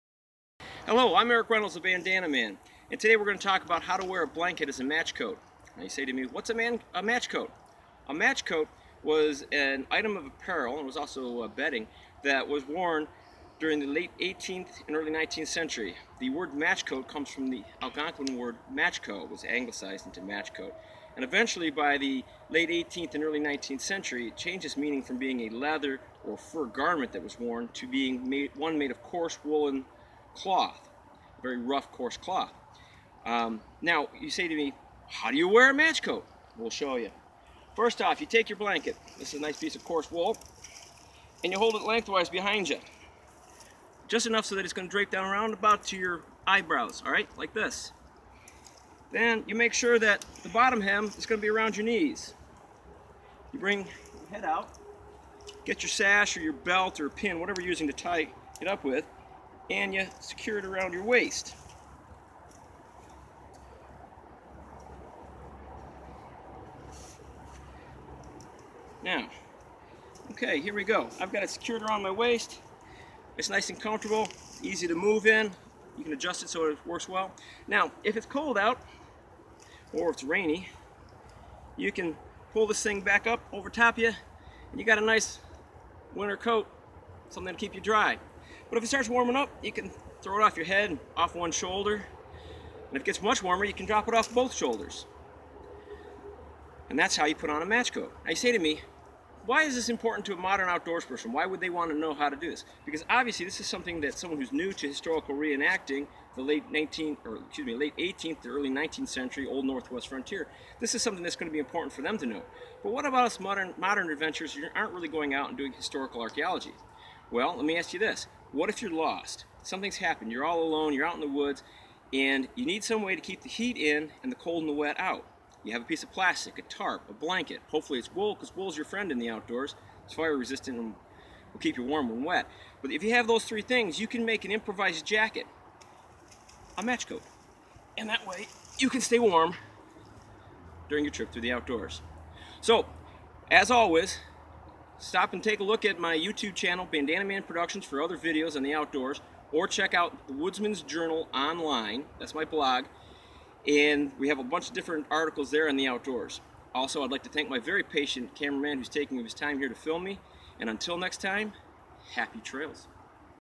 How to Wear a Blanket as a Matchcoat
Have you ever wondered what a matchcoat was or how to wear it?  Do you know how to improvise a jacket or a raincoat out of a piece of plastic, a tarp or a blanket?  Watch this video and find out.